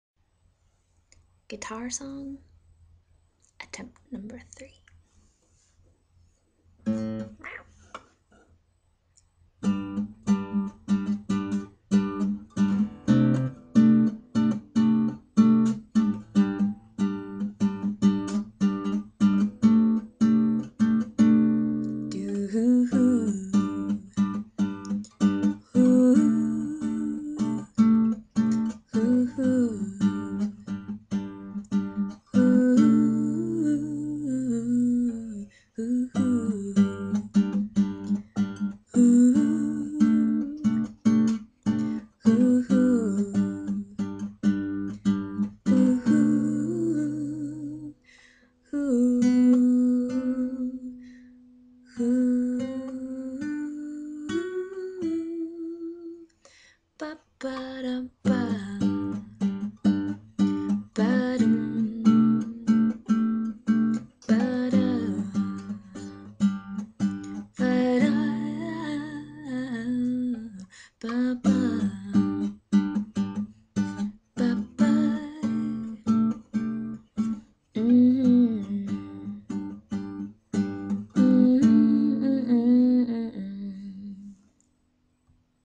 ModifiedG.wmv
Just chords and simple melody. Chorus tune? Need lyric ideas.